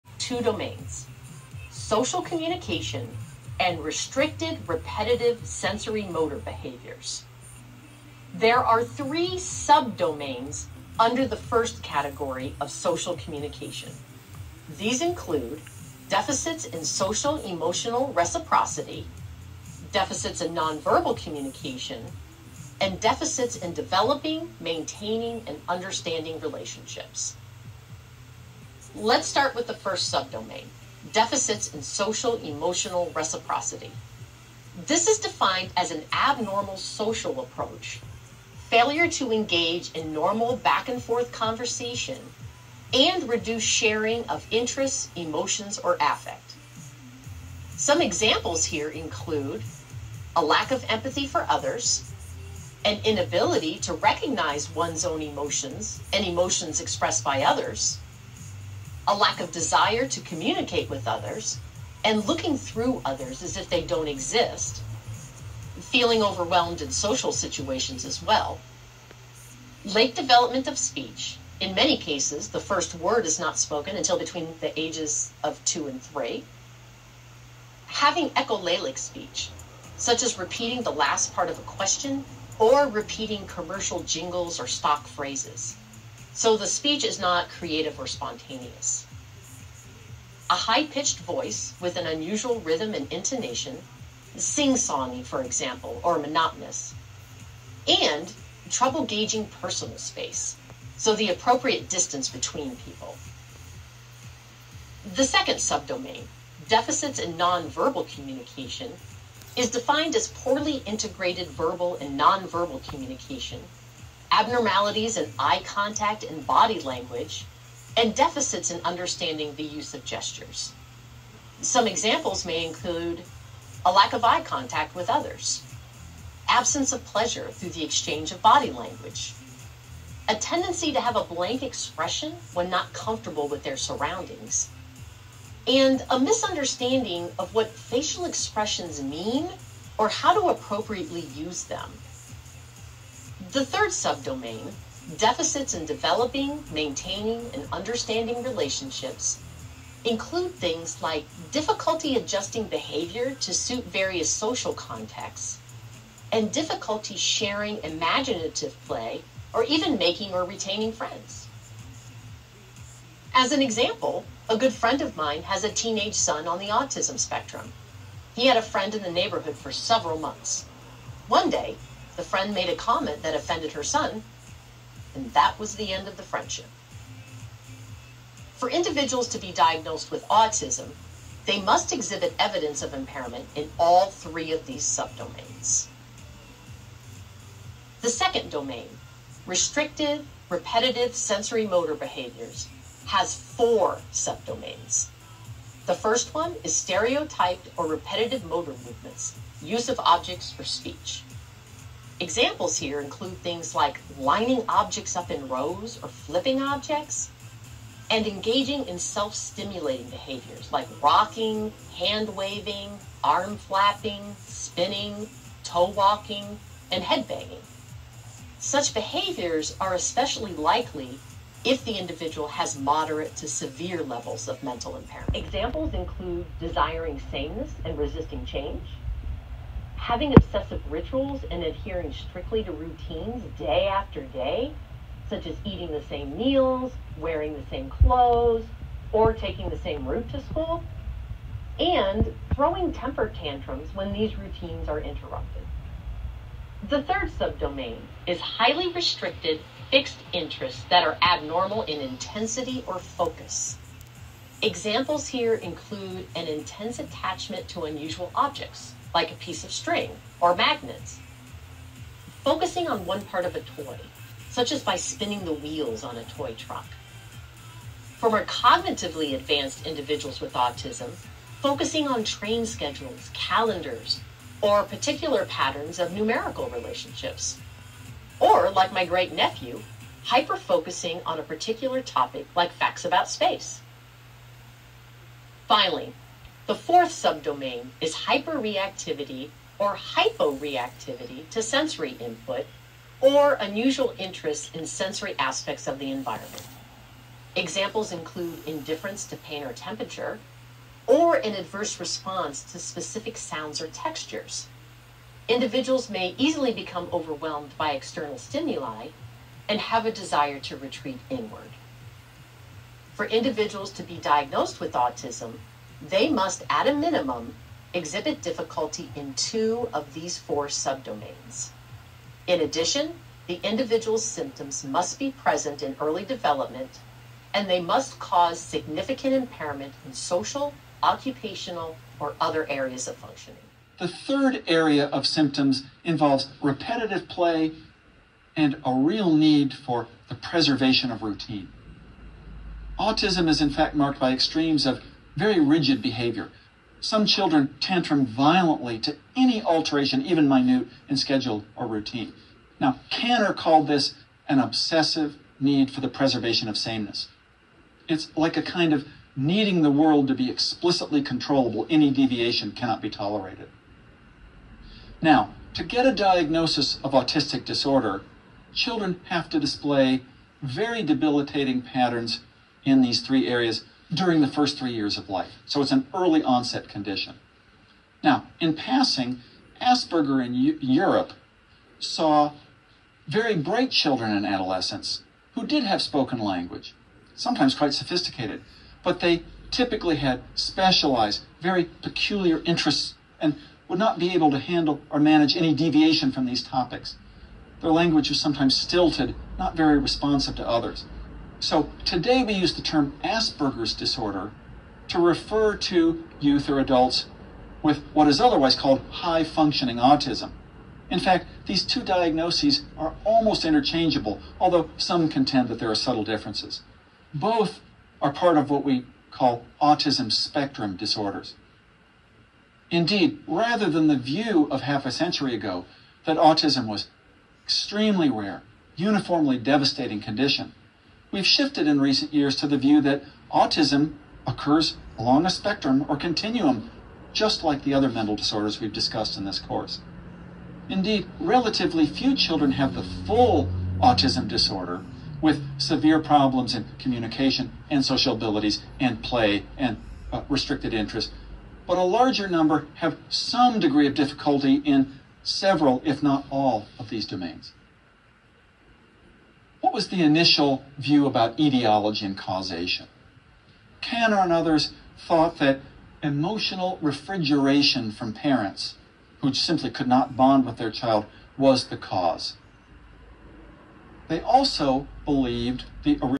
World Autism Awareness Day
Today is World Autism Awareness Day 🧩 ❤️🌹 our science advances more and more understanding neuroscience and diversity 💐🪴👩‍🌾 fascinatingly, autism is identified by behaviour only, no other diagnostics exist yet! 💙💚💛🧡